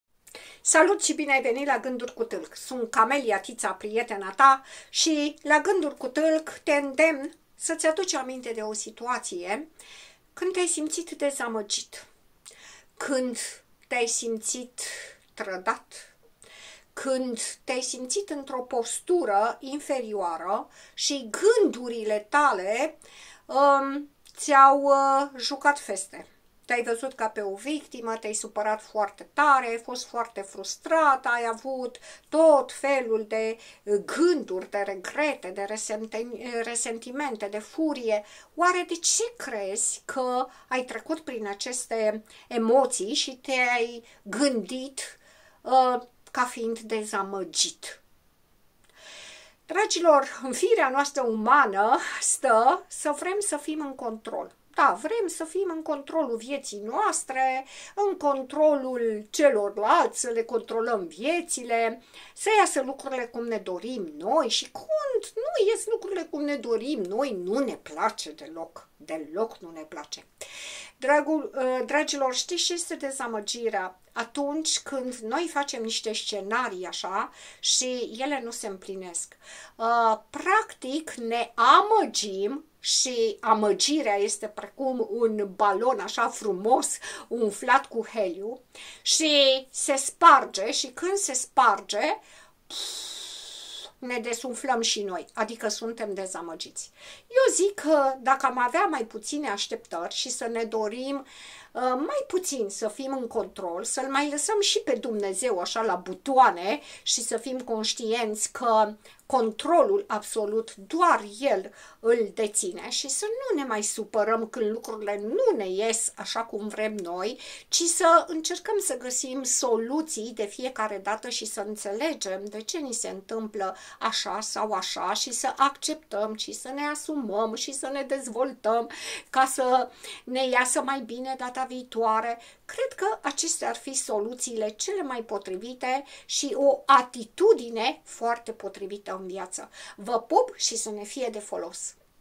282.,,De ce ne simțim dezamăgiți?"-Ganduri cu tâlc, 24.01.2023
282.,,De ce ne simțim dezamăgiți?"-Ganduri cu talc, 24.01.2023
Suntem dezamăgiți când ne amăgim singuri și din orgoliu, credem că deținem controlul.